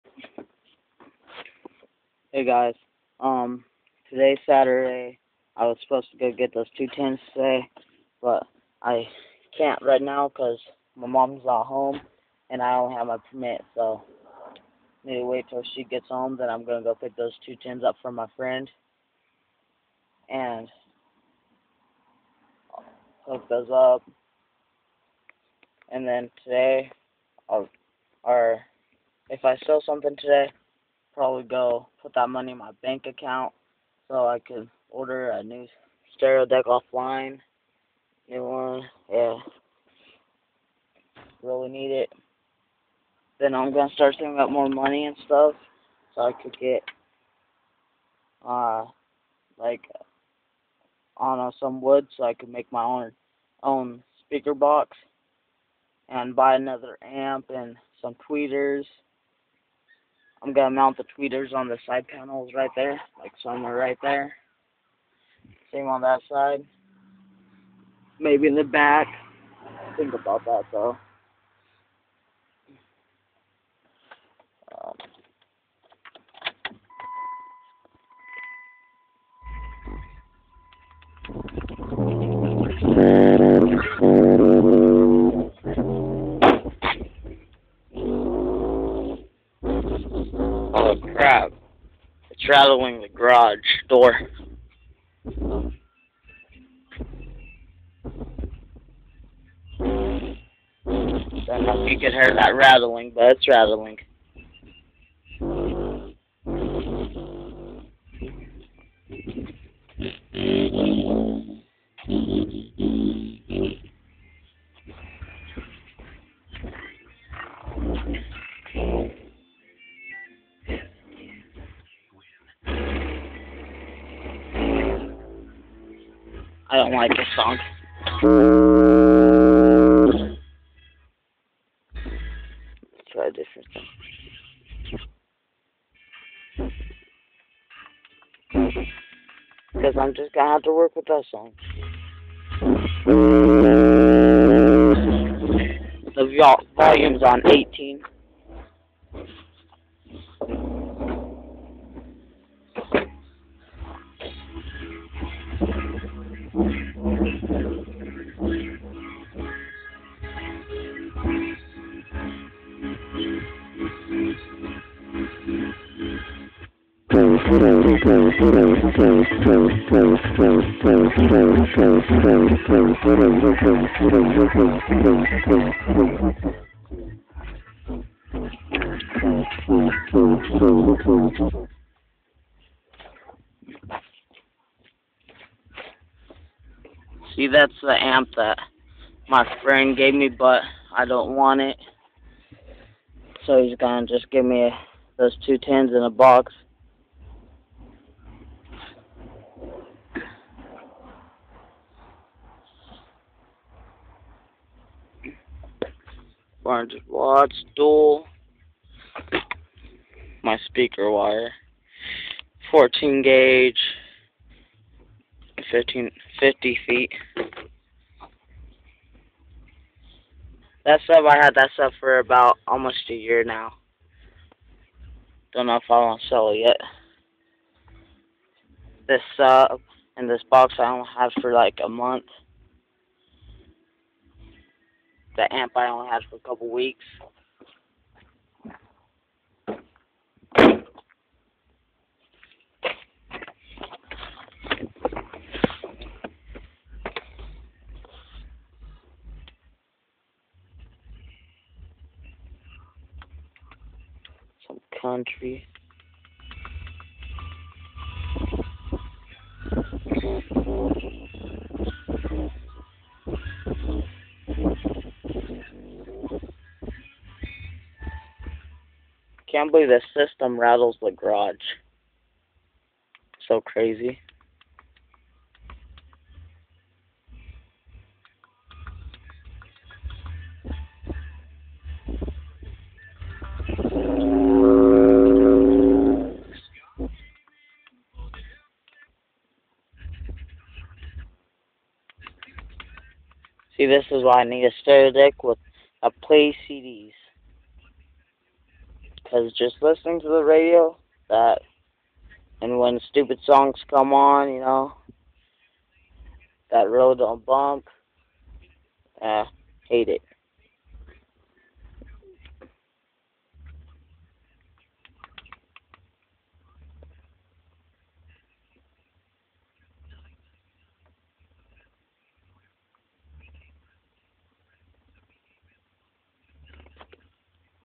10" sub hiting hard
the sub is shaking the garage door